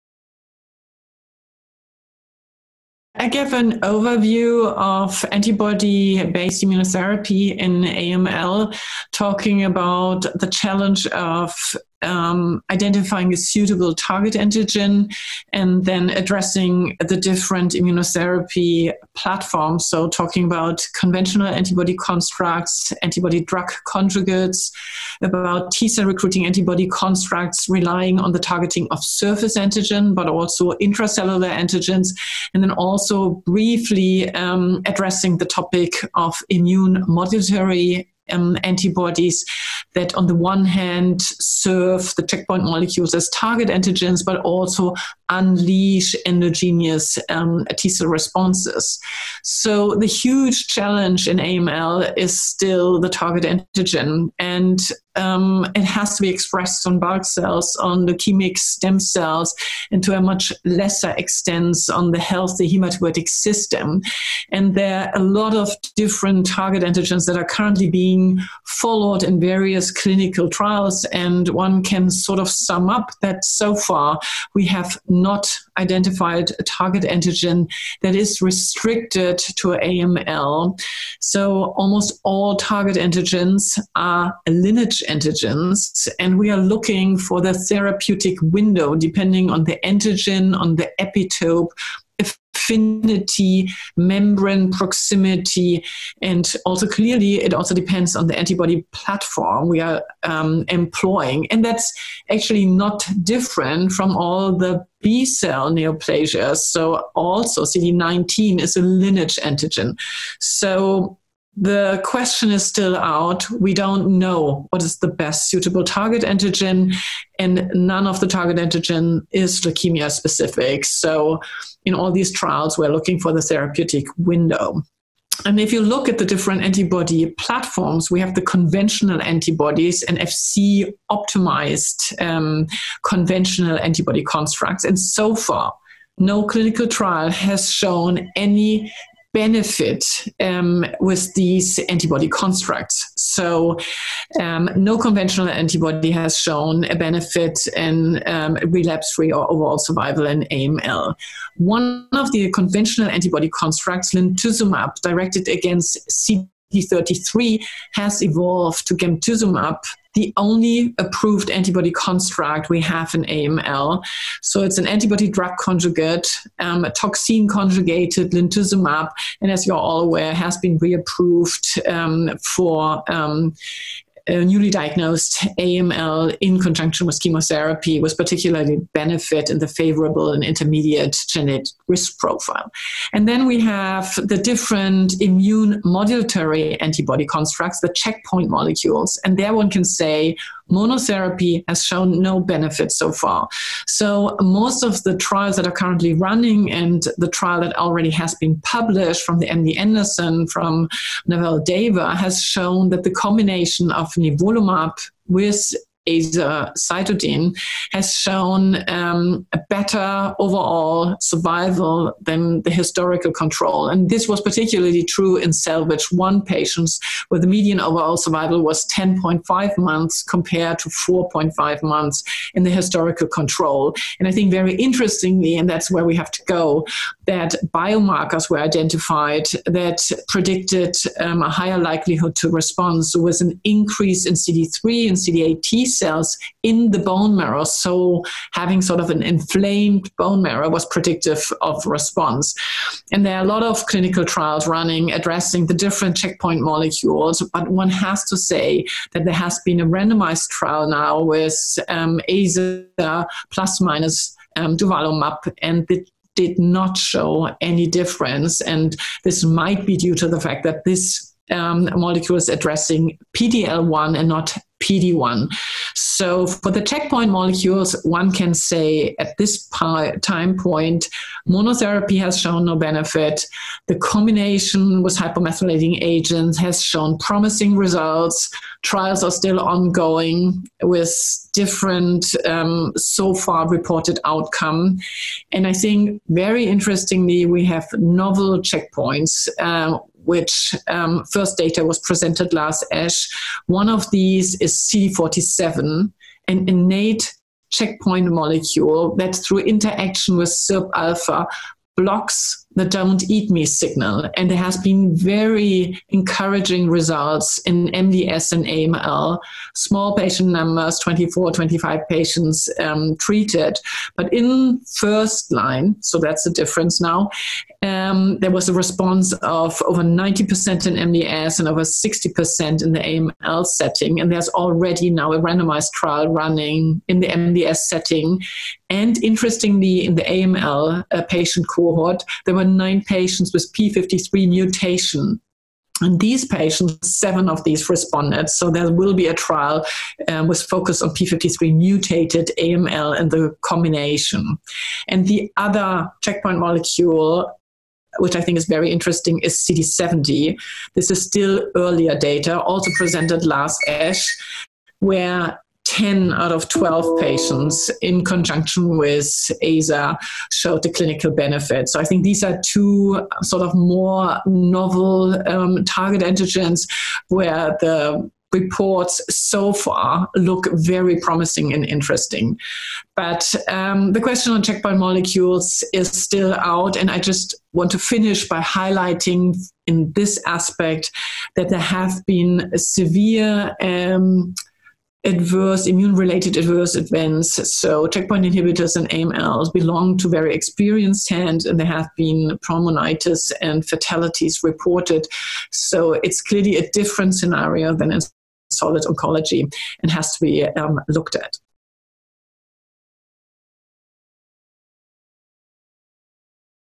Novel antibodies in AML
Marion Subklewe, MD, Ludwig-Maximilians-University of Munich, Munich, Germany, outlines novel antibody-based immunotherapies developed for the treatment of acute myeloid leukemia (AML), as well as challenges with implementing these agents. Prof. Subklewe also discusses the development of gemtuzumab, the only antibody-drug conjugate approved for the treatment of AML as well as checkpoint inhibitors such as nivolumab, that have been investigated in monotherapy and combination in this indication. This interview was recorded via an online conference call with The Video Journal of Hematological Oncology (VJHemOnc).